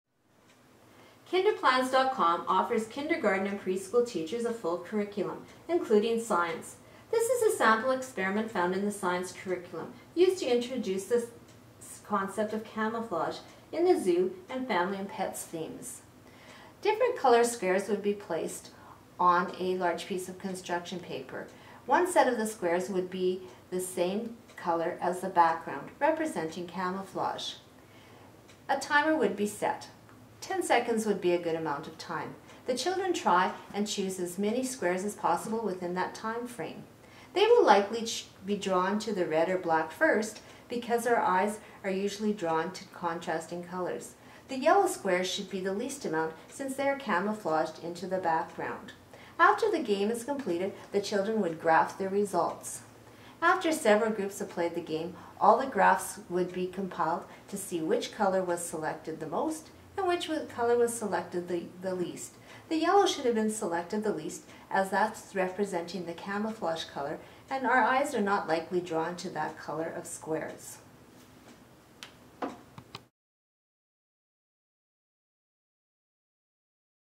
Science-Experiment-Camouflage.mov
Preschool and Kindergarten science experiment to help develop the concept of camouflage.

Kinderplans.com